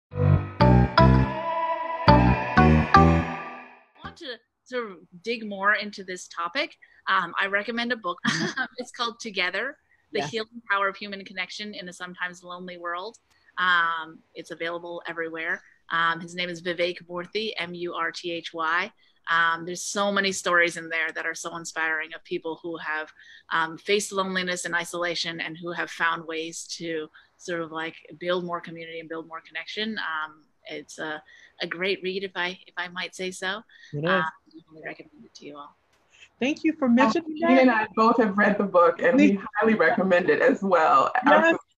Recommended Reading on Overcoming Loneliness and Isolation - Dr. Vivek Murthy's, Together
IAWH talked with Dr. Alice Chen, an internal medicine physician, who helped to define the differences between loneliness and isolation and offered tips for navigating both. During the peak of the COVID-19 pandemic, various factors such as personal circumstances, lifestyle, and social connections influenced whether people felt isolated. Some individuals felt socially connected and engaged, while others experienced isolation due to geographical distance, busy schedules, or a lack of close relationships. Dr. Chen provided some suggestions for balancing our lives by identifying ways to increase socialization and find the good in gathering with other people while embracing time to ourselves, which is also critical to our personal growth and development.

Connect with IAWH: instagram: @advancingherhealth; facebook: @advancingherhealth; X (formerly Twitter): IAWH_HerHealth